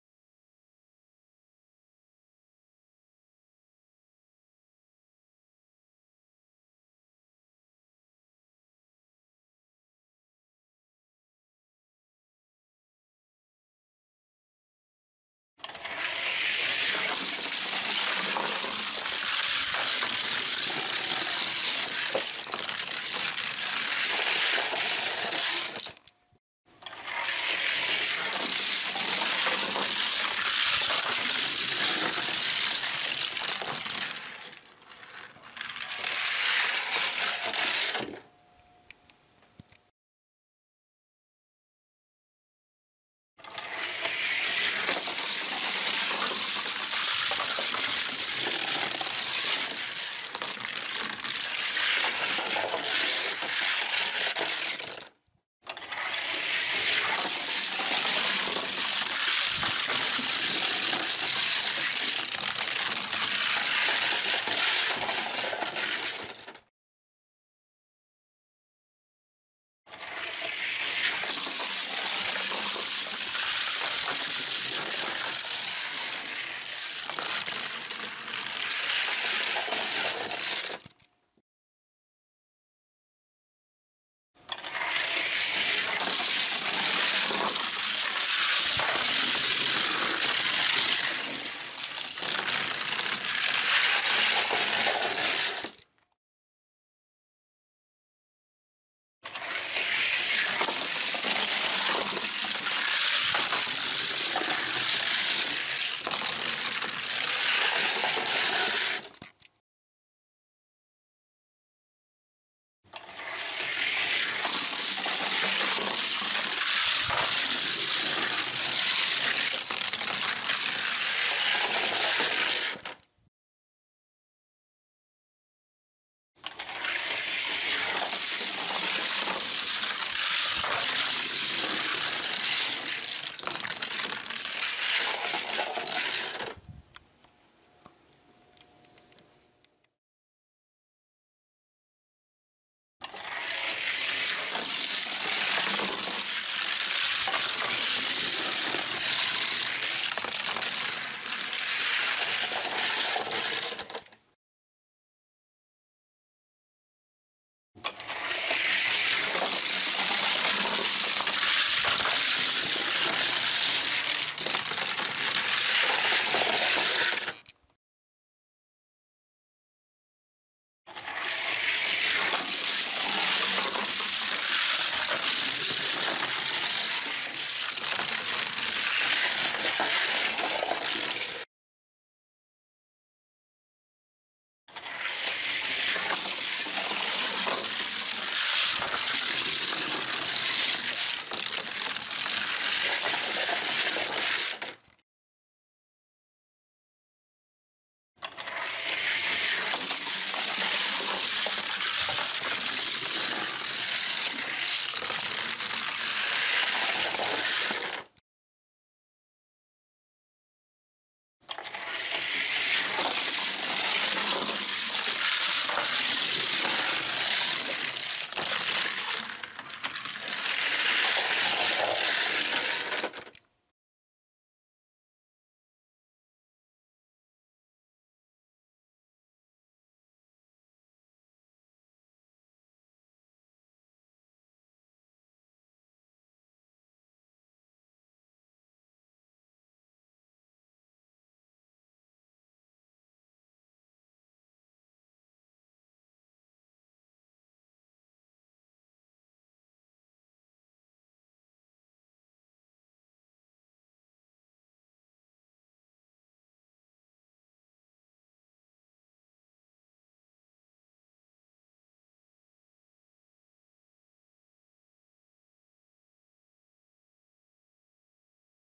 GT40 vs. GT Racer - Race 4 - Group 8 - DCR 2021 Formula R Challenge
Event:  DCR 2021 Formula R Challenge (Group8)
Round:  1  Race: 4
Cars:  HW Ford GT40 (blue) vs. HW GT Racer (white)
Teams:  Track Cats vs. Pocket Rockets